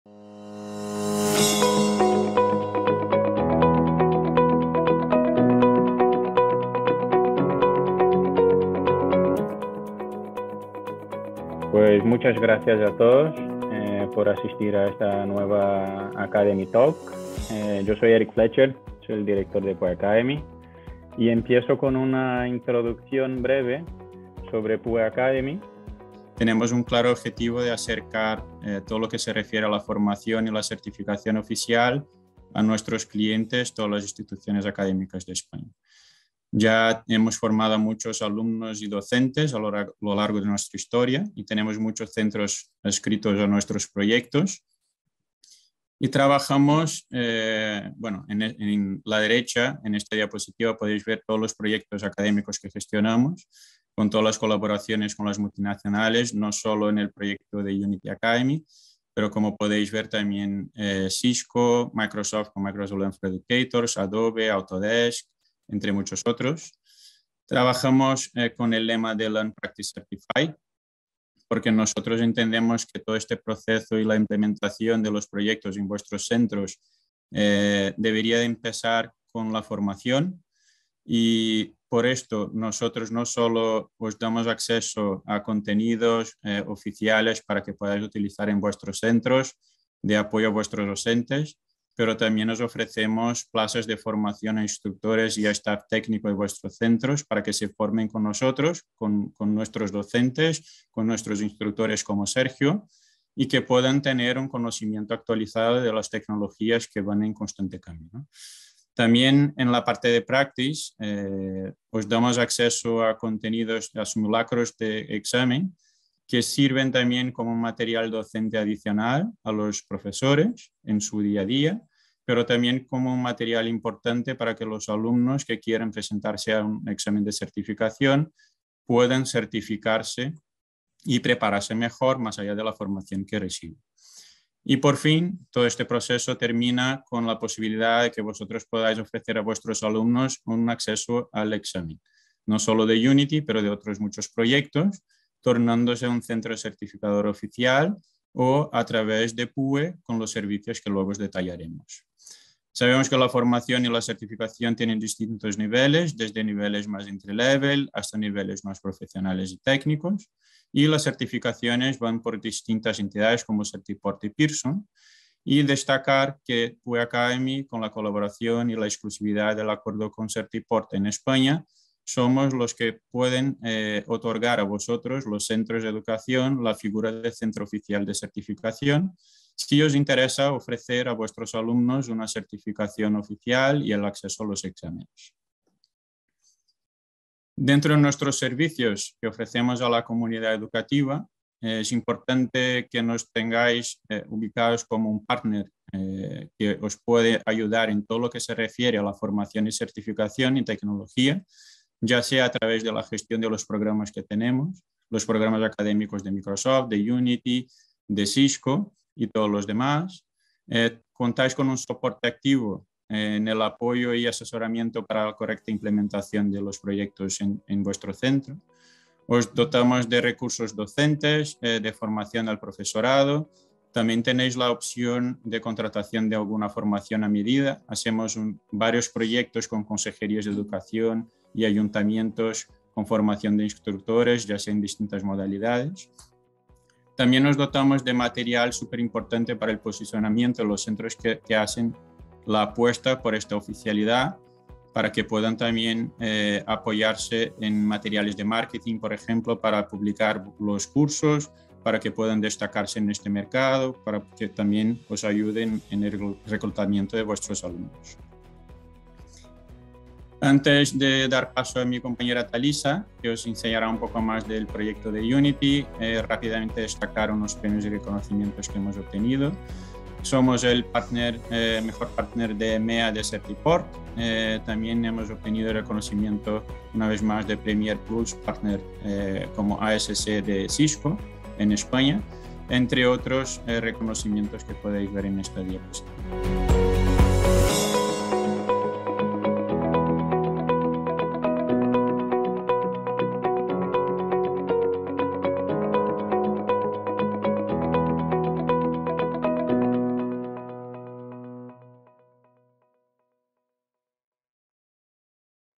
Webinar Forma y Certifica en videojuegos con Unity - Bienvenida y presentación
Conoce el universo educativo de Unity para formar a tus estudiantes con contenido oficial y darles la posibilidad de certificarse en una industria en auge.

En este vídeo, Eric Fletcher, Director de PUE Academy, presenta el webinar y hace una breve presentación de los servicios de PUE Academy a la comunidad educativa, como partner en todo lo relativo a formación y certificación oficial en tecnología: soporte y asesoramiento en el despliegue de los programas académicos de las multinacionales líderes del sector IT, recursos docentes, formación de instructores, formación a medida, recursos de MK para el posicionamiento de los centros y reclutamiento de alumnos.